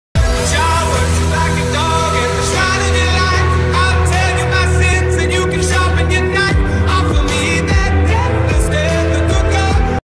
New intro look in description for info
I'm uploading this new intro hopefully I can use it by I'm not sure I did a intro last time but I could not use it I'm sorry I'm not online or have uploaded in a while I'm be back soon hopefully when I get back we are starting somthing new I have vr now so expect a lot of vr videos hope you enjoy again I will try to use this intro